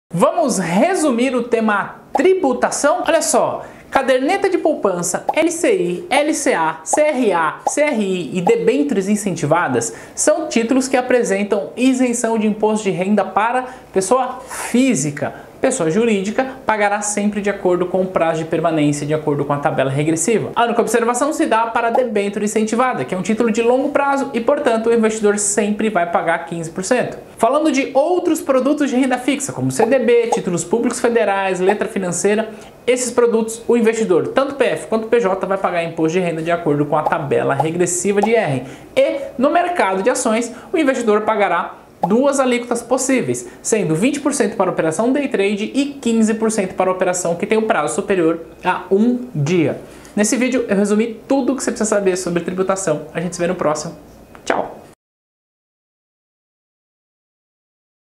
Um minuto de... Tributação
No primeiro vídeo da nossa nova playlist, vamos resumir tudo o que você precisa saber sobre Tributação em UM MINUTO! Aproveite e use essa playlist para ajudar nos estudos ;)

Quer conhecer nossos cursos completos?

Estude com quem realmente aprova!